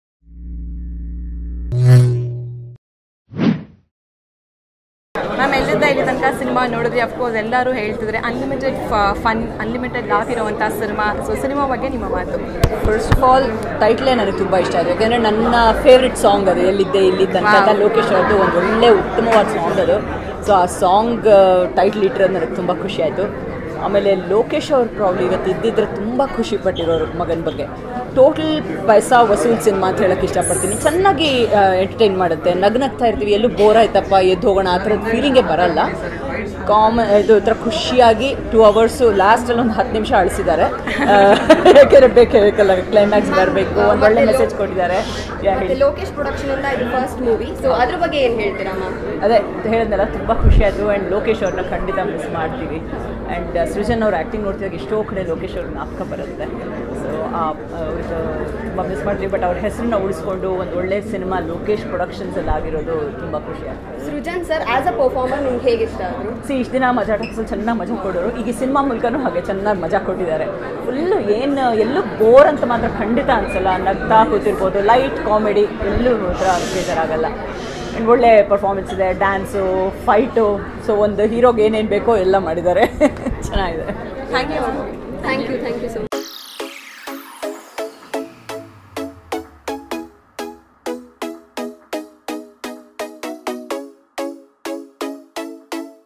ಲೋಕೇಶ್ ಅವರು ಇವತ್ತು ಇದ್ದಿದ್ರೆ ಮಗನ ಬಗ್ಗೆ ತುಂಬಾ ಖುಷಿ ಪಡ್ತಿದ್ರು | Suman Nagarkar | Srujan Lokesh Films
Actress Suman Nagarkar Speaks About Ellidde Illi Tanaka Movie Celebrity Show Maja Talkies Srujan Lokesh Haripriya New Film Ellidde Illi Tanaka Songs Trailer